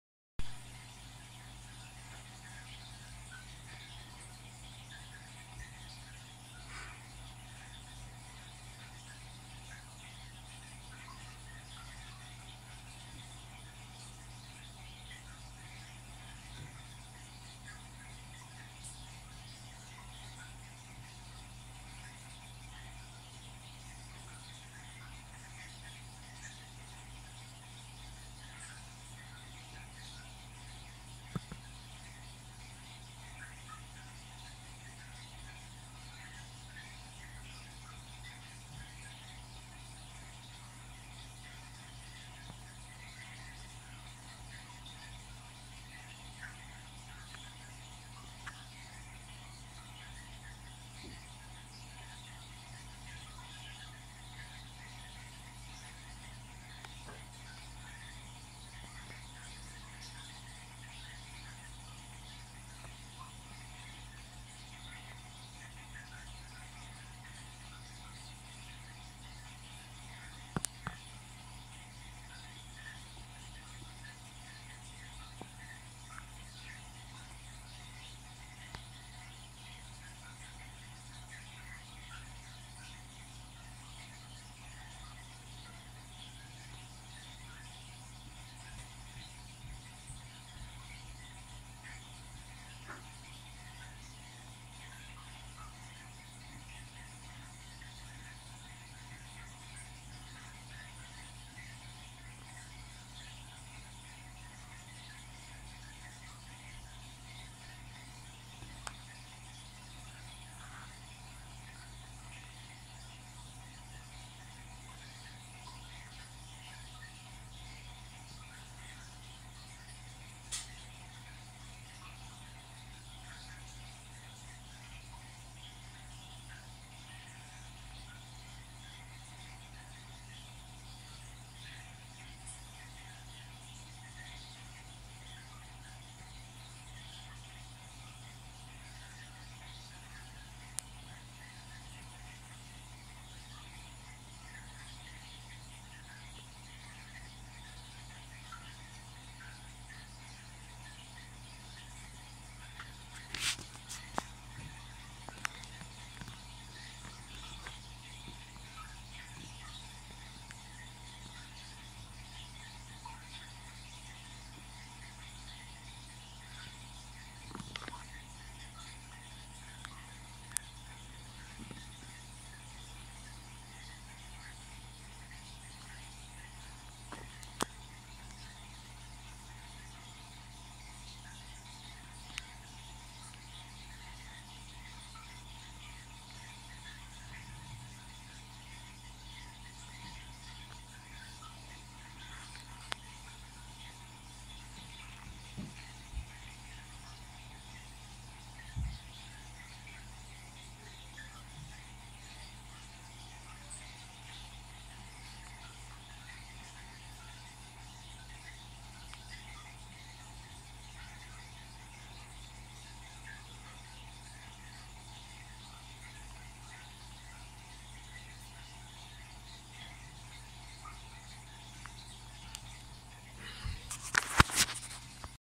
Microgeophagus ramirezi Einzug.
Einzug von 2 Pärchen Microgeophagus ramirezi ins 756 Liter Südamerika Becken.